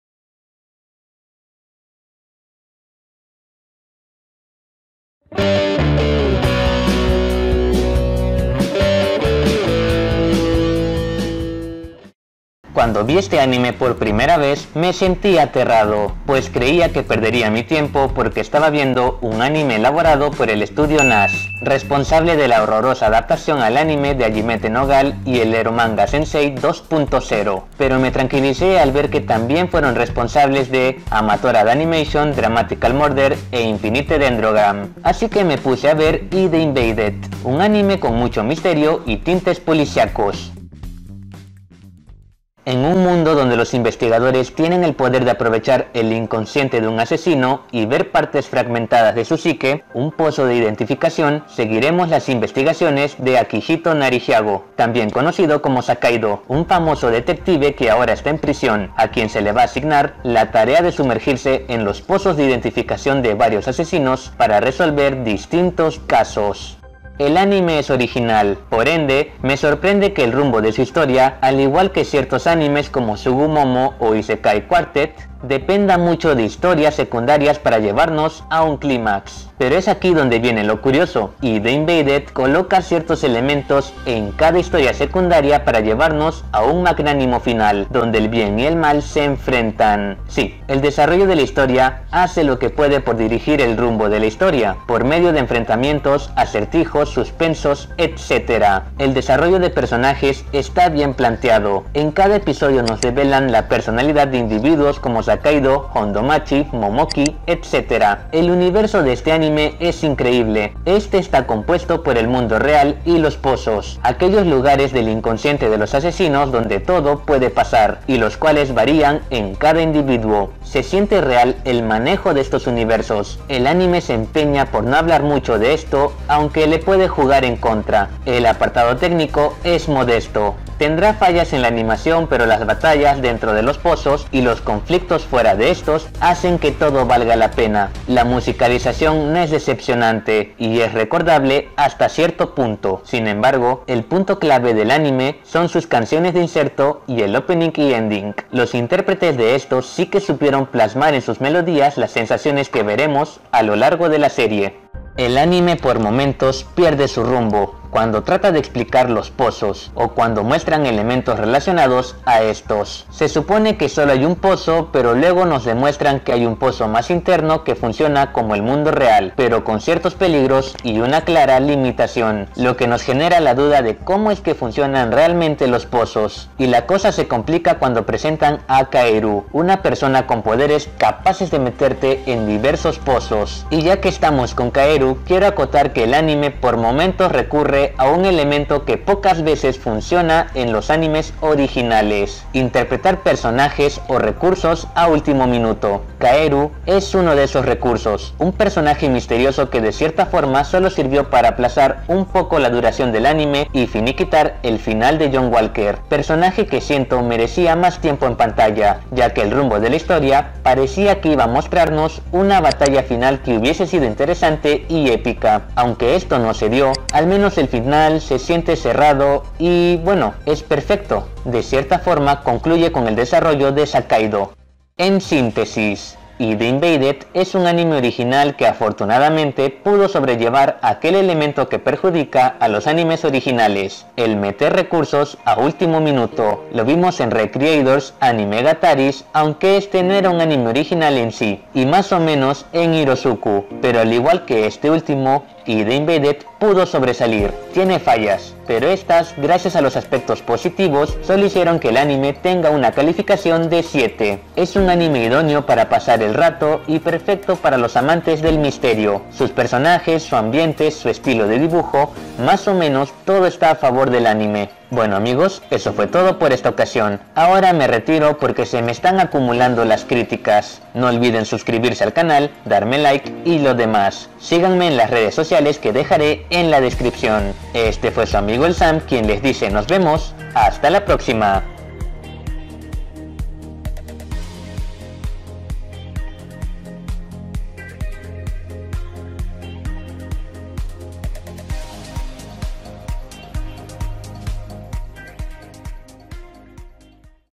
ID: INVADED | Un peculiar viaje al inconsciente
¿Qué obtienes cuando mezclas el psicoanálisis de Freud y la ética de Rousseau con una historia policíaca ambiciosa? Pues obtenemos al infravalorado ID: Invaded, un anime que tiene ciertas falencias pero su historia lo compensa.

Sigan a Avii por Instagram, ella es la mente maestra detrás del fantástico perfil del canal.